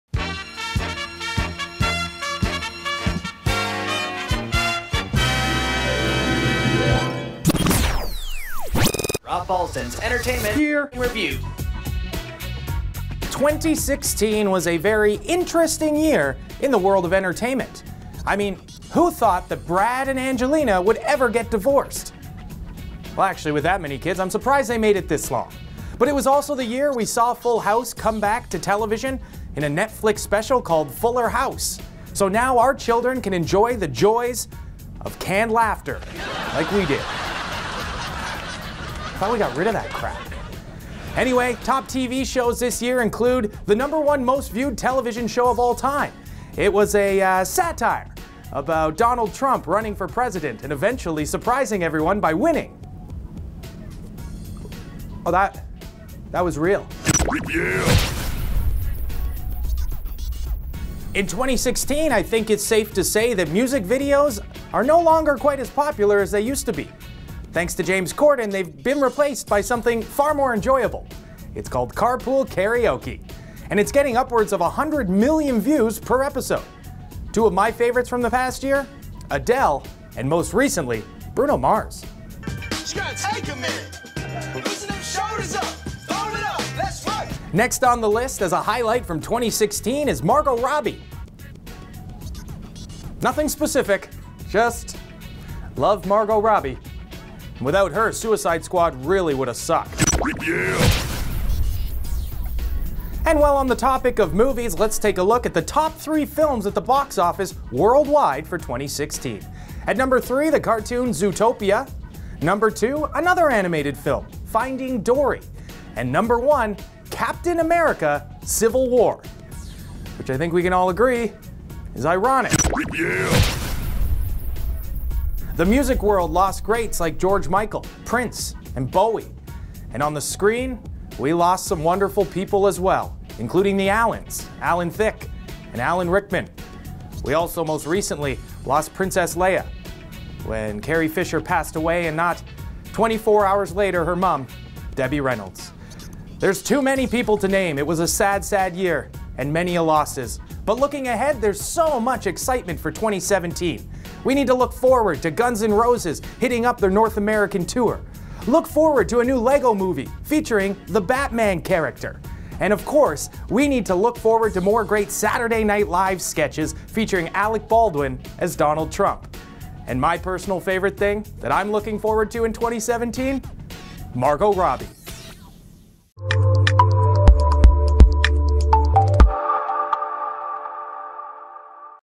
Entertainment Year in Review
It's always busy in the entertainment world and Rob Balsdon has all the goods on what happened this past year.